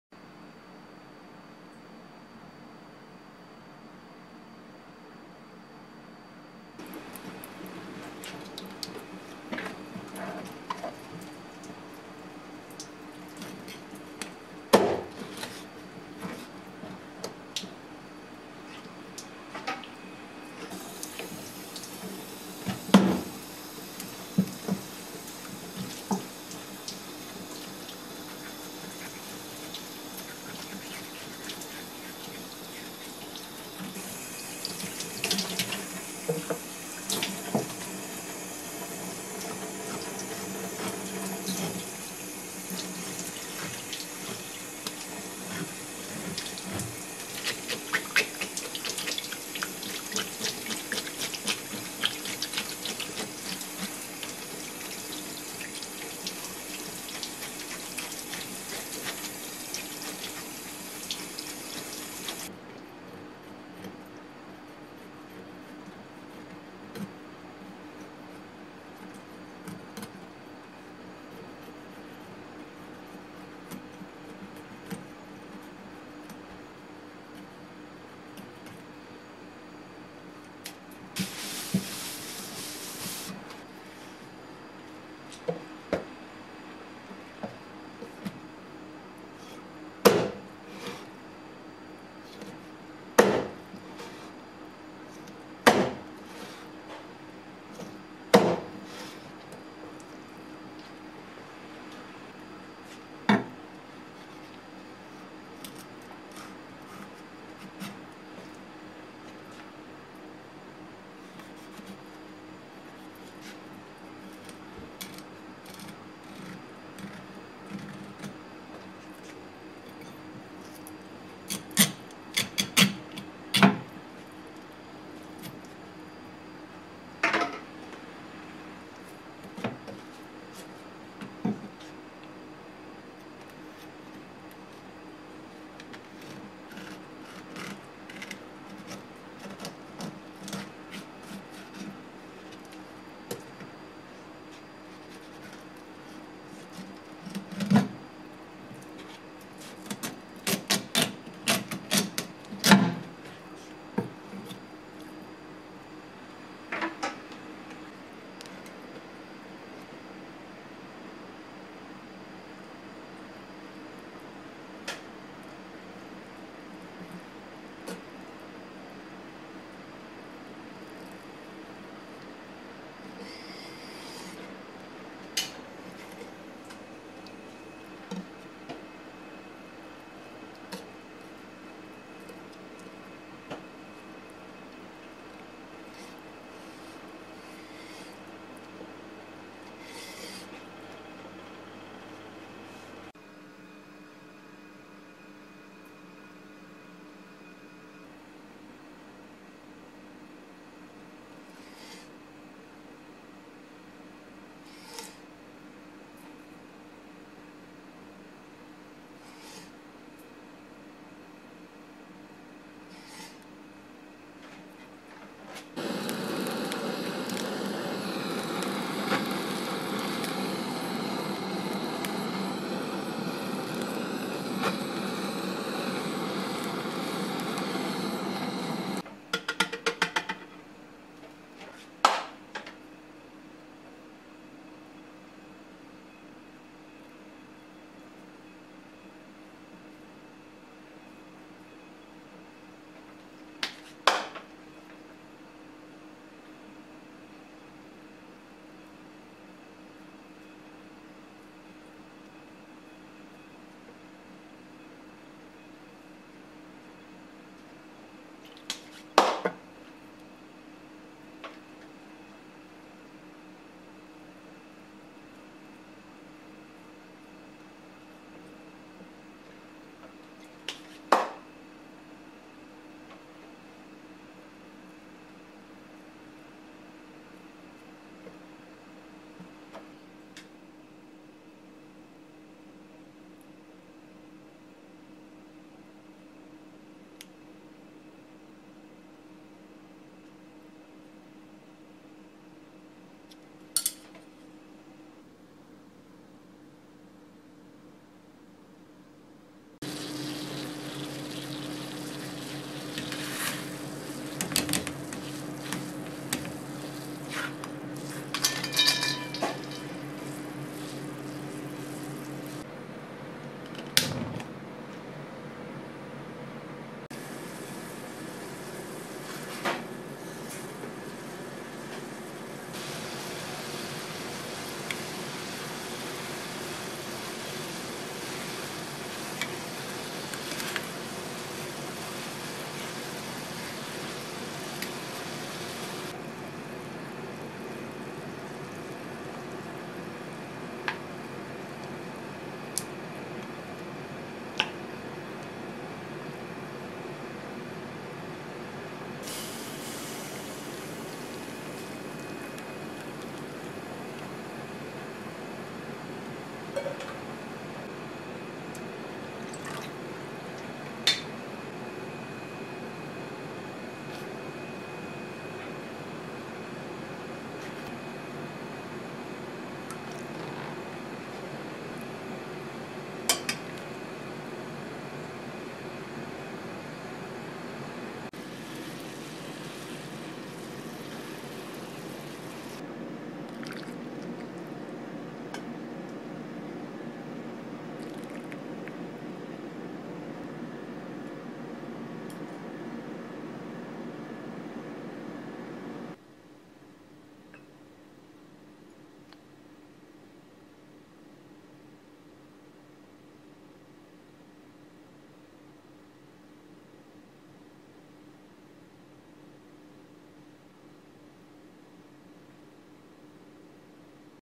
ヤガラのさばき方～握り寿司と吸い物の作り方～how to fillet a Smooth flutemouth and make sushi and soup
寿司屋の仕込みの一部始終を動画で紹介しています